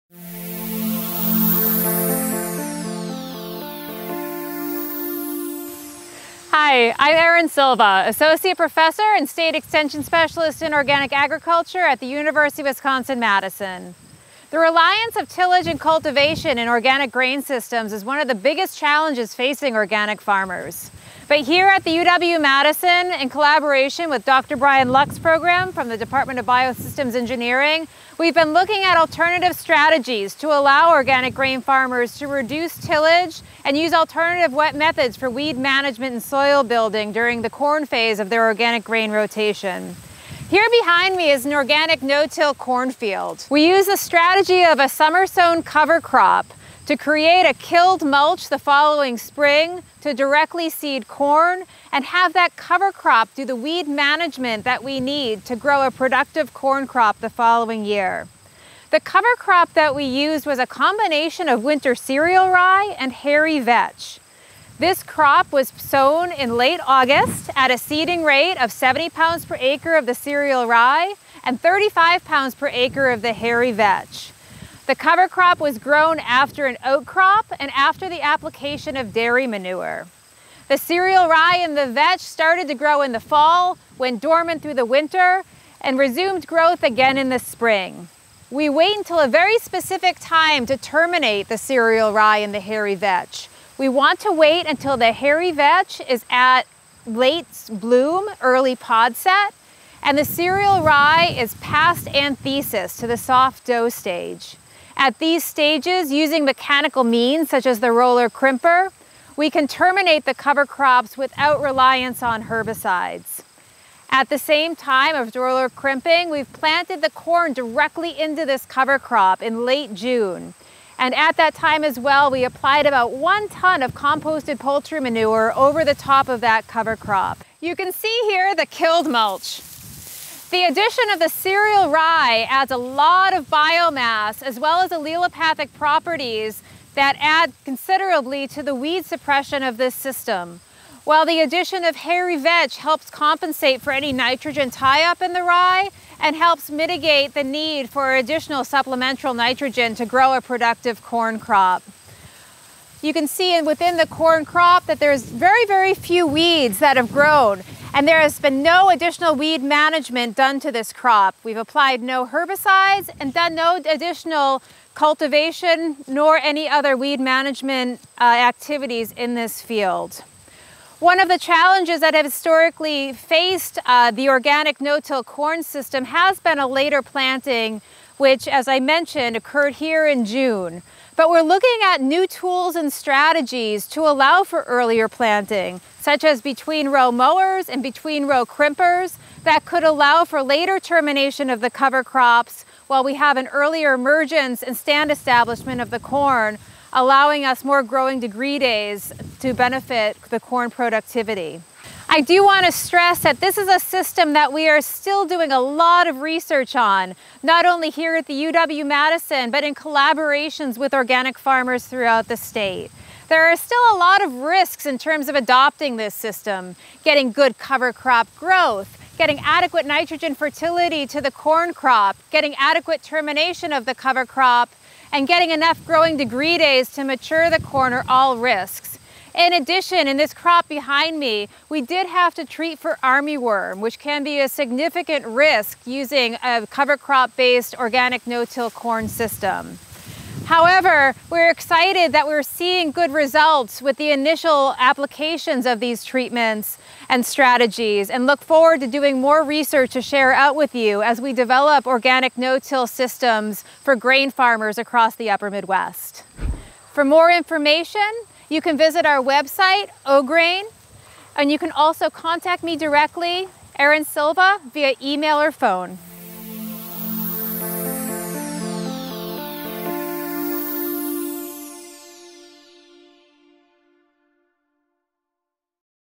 Organic No-till Corn Into Cereal Rye and Hairy Vetch Cover Crop
Dr. Erin Silva, UW-Madison, takes you into a research field of organic no-till corn. It was planted into terminated cereal rye and hairy vetch cover crop which provided the season long weed suppression. Dr. Silva discusses the inputs used and the challenges faced during the season.

and UW-Madison Integrated Pest Management and Nutrient & Pest Management